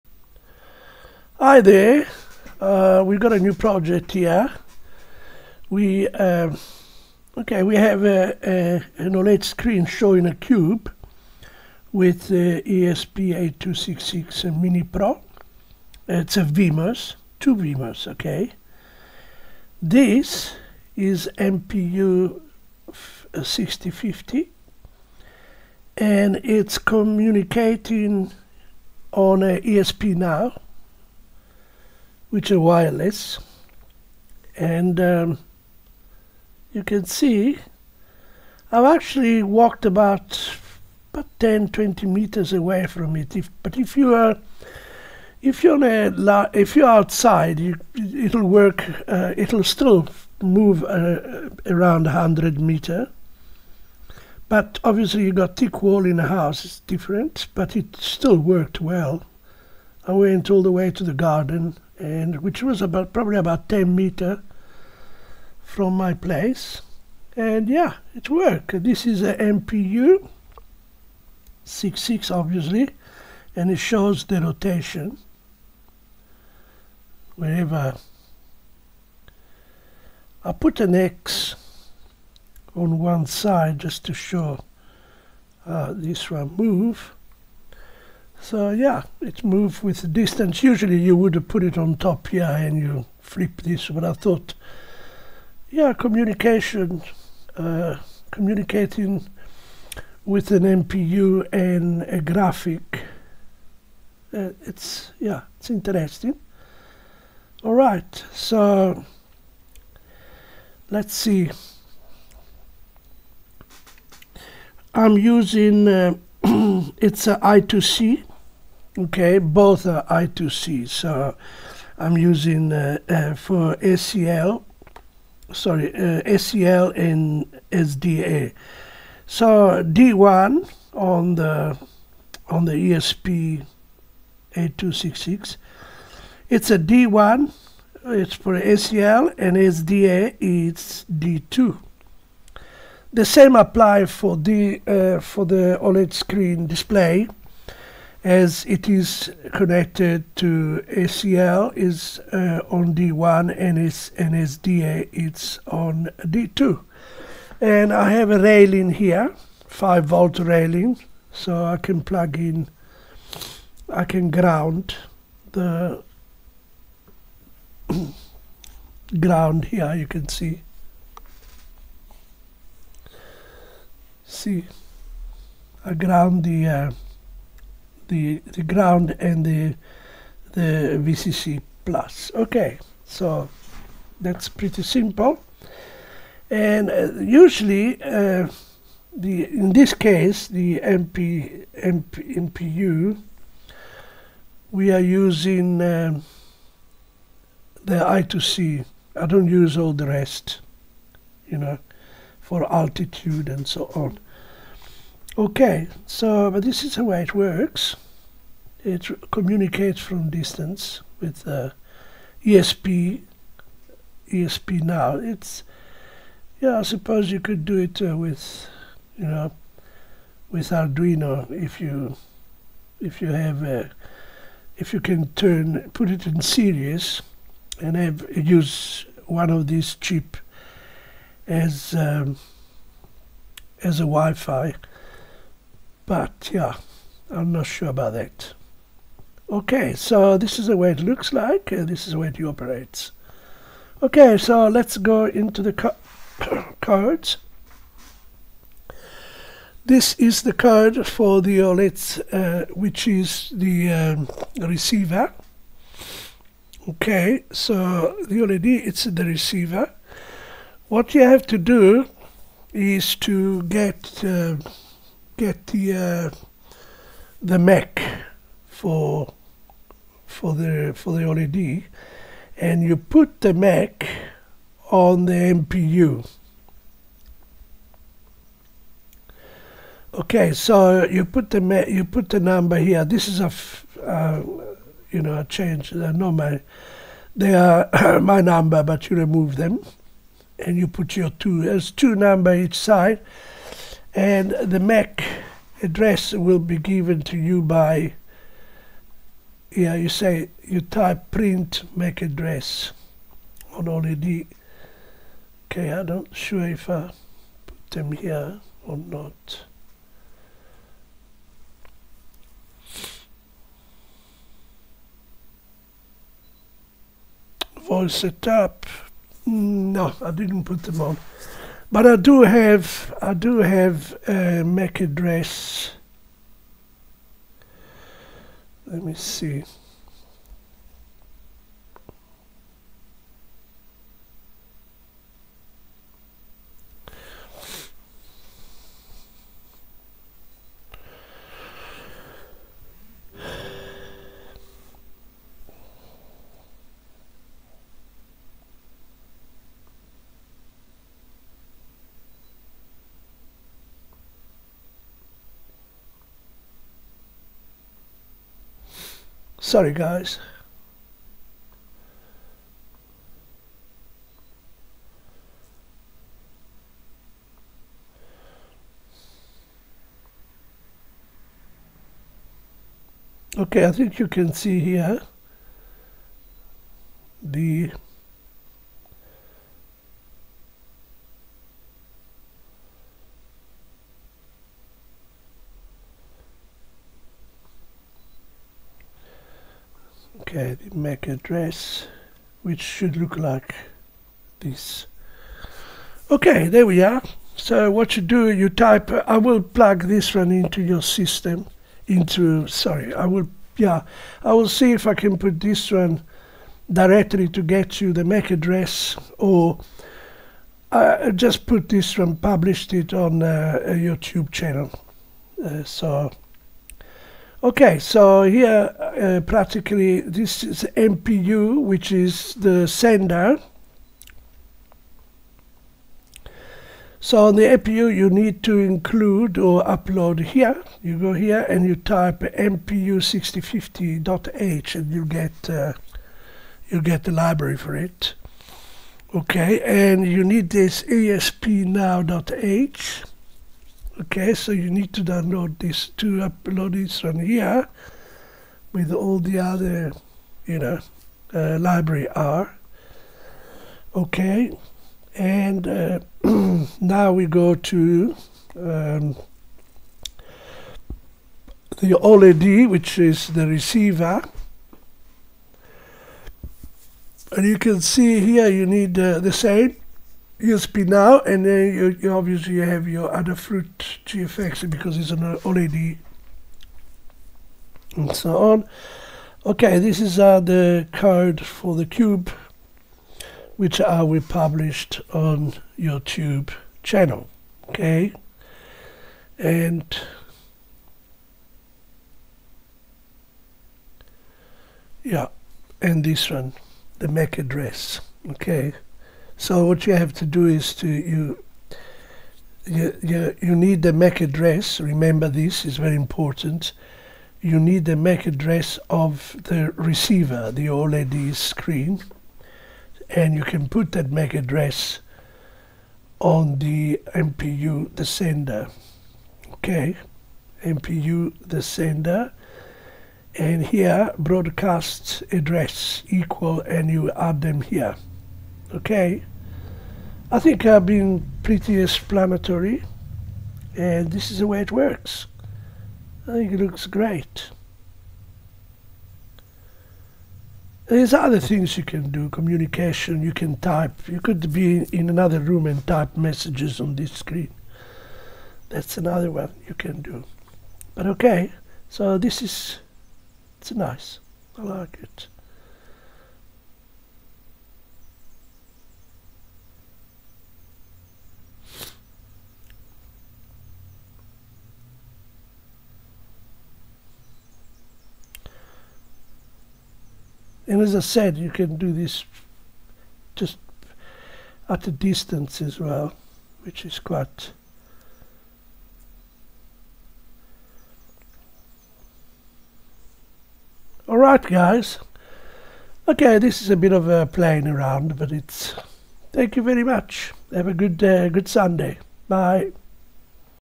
ESP-NOW transmit real-time orientation data from an MPU6050 motion sensor to another
3D Cube Orientation Visualizer via ESP-NOW
Objective:
To transmit real-time orientation data from an MPU6050 motion sensor to another WeMos board, and visualize this motion as a rotating 3D cube on an OLED display—wirelessly using ESP-NOW.
Hardware Used:
• 2 × WeMos D1 Mini Pro (ESP8266)
• 1 × MPU6050 (Gyroscope + Accelerometer)
• 1 × SSD1306 128×64 OLED display (I²C)
• Jumper wires, optional antenna for improved range
 Architecture Overview:
1. Sender Node:
o Reads orientation (pitch/roll) from MPU6050 via I²C.
o Formats and transmits this data using ESP-NOW to the receiver.
o Keeps the loop light to allow fast and frequent updates.
2. Receiver Node:
o Listens for ESP-NOW packets.
o Parses pitch and roll angles.
o Uses this data to draw a simple 3D cube with a marked face (X) on the OLED.
o The cube rotates according to sensor movement.
How It Works:
1. MPU6050 Initialization (Sender):
o Sensor is calibrated and initialized using Wire and MPU6050 libraries.
o Pitch and roll angles are calculated based on accelerometer data.
2. ESP-NOW Communication:
o Sender and receiver are initialized as ESP-NOW peers.
o The sender transmits pitch and roll values in a small payload (e.g., struct with 2 floats).
o Receiver receives this payload without needing Wi-Fi connection or router.
3. 3D Cube Rendering (Receiver):
o Receiver reads the incoming angles.
o It maps these to 2D projection (basic, no perspective).
o The cube is drawn on the OLED with one face marked using an X so the user can see orientation.
o Frame is updated continuously for smooth effect.
 Why ESP-NOW?
• Fast (much faster than traditional Wi-Fi client/server).
• Low Power
• No Wi-Fi Router Needed
• Perfect for sensor-to-display communication
 Use Cases:
• Simple motion visualizer
• Wireless input device
• Orientation feedback for remote-controlled devices or robots